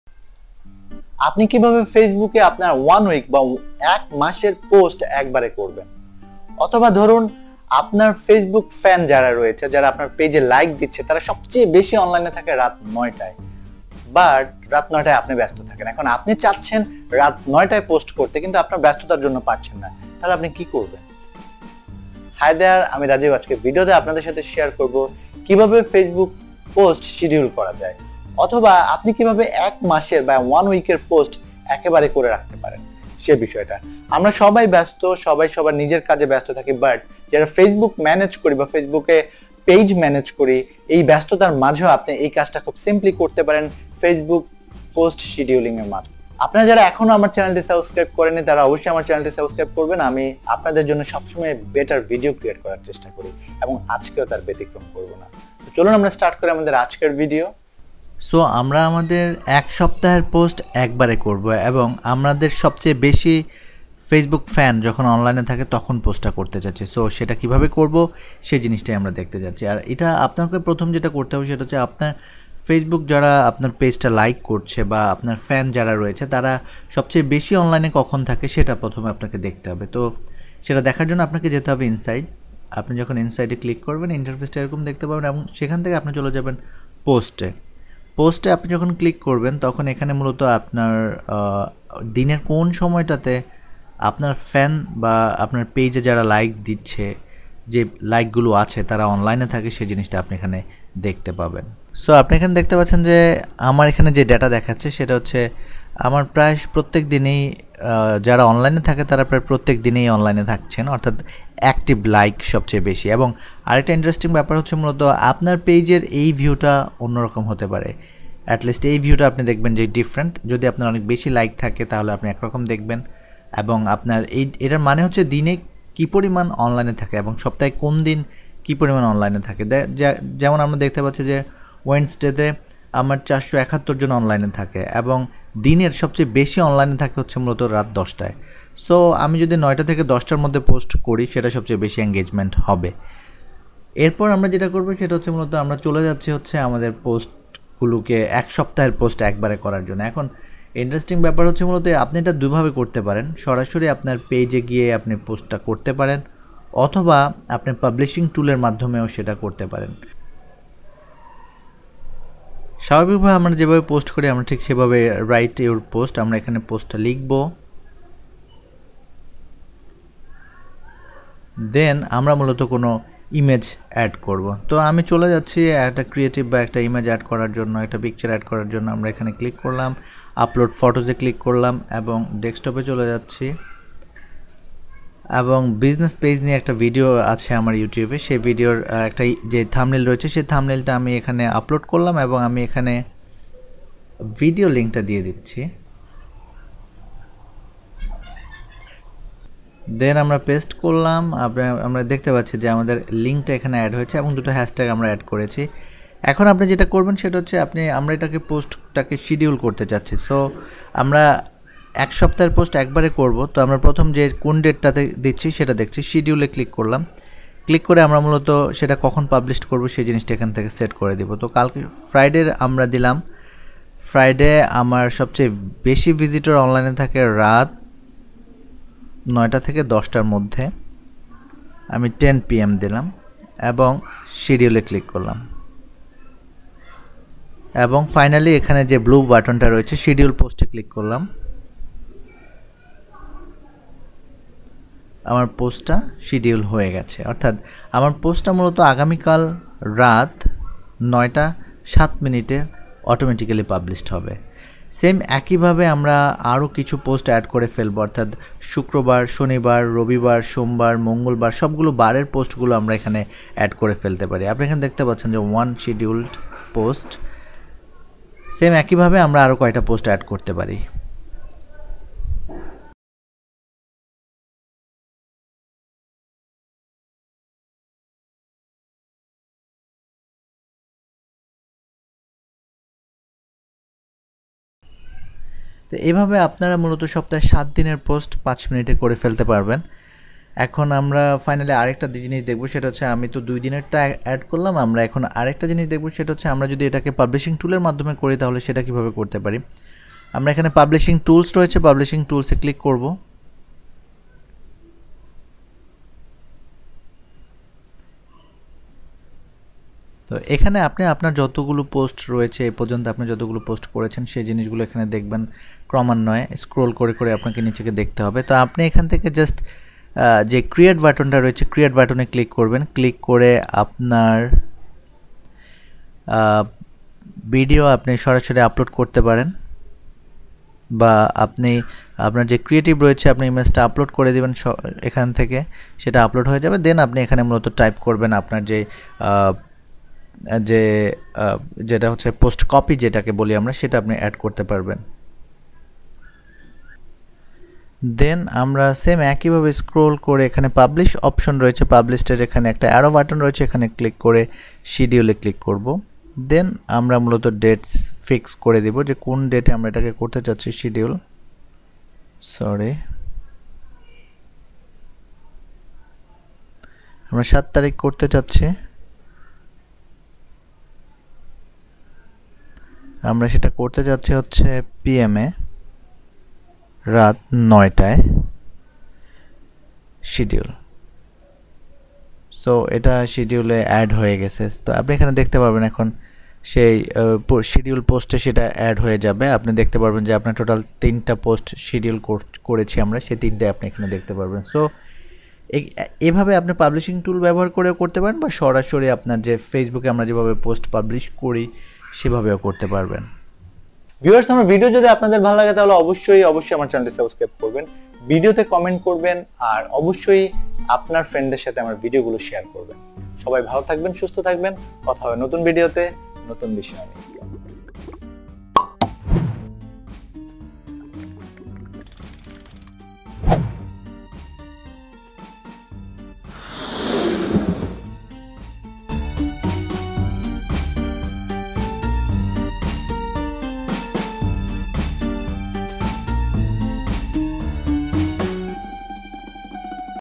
How To Use Facebook Post Scheduled - Bangla Tutorial
This video will teach you how to use facebook post scheduled features - facebook marketing Bangla tutorial